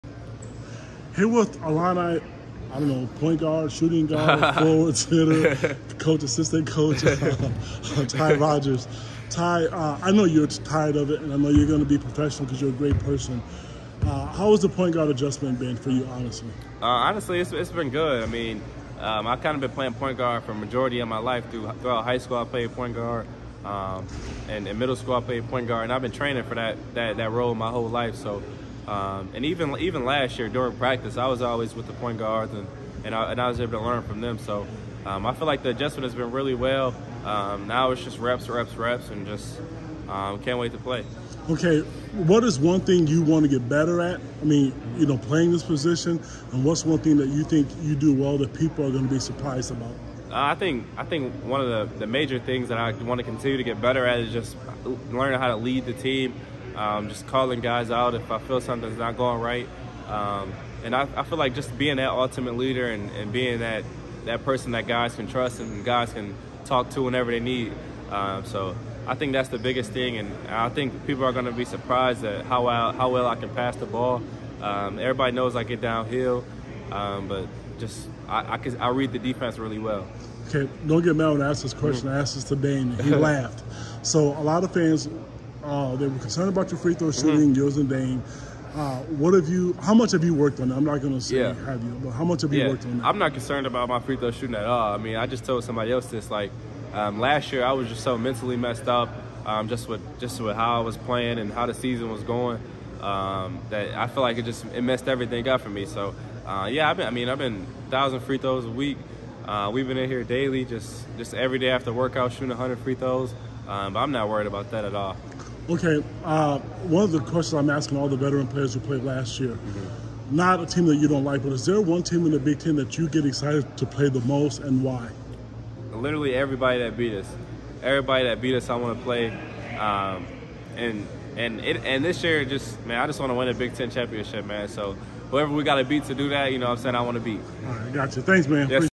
Illinois Ty Rodgers media day 9/29/23
Talks during media day and his adjustments to moving to point-guard full time.